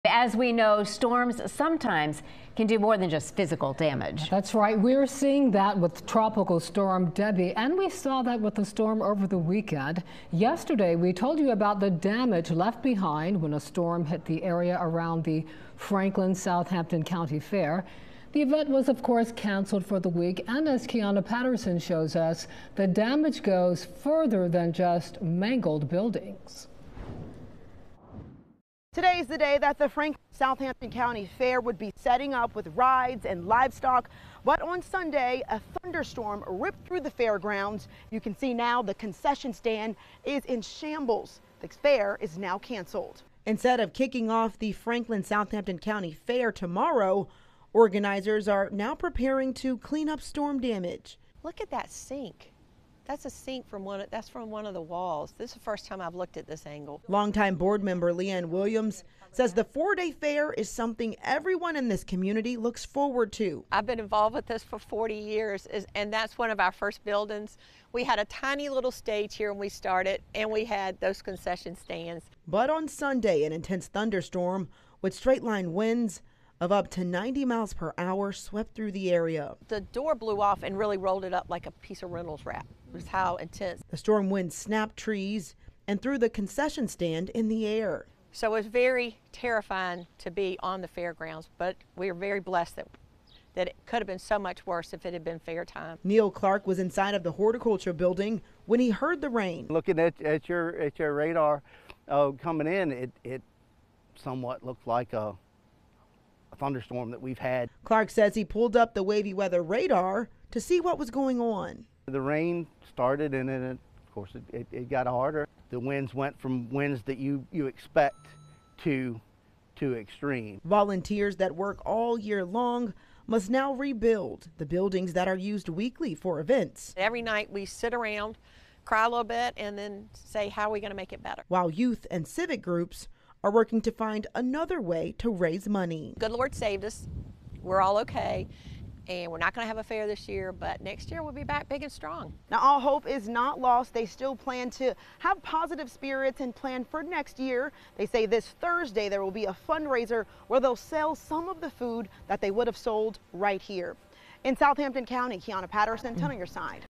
'The good Lord saved us. We're all okay.' Franklin-Southampton County Fair canceled following Sunday
'The good Lord saved us. We're all okay.' Franklin-Southampton County Fair canceled following Sunday storm
Kiahnna Patterson reports.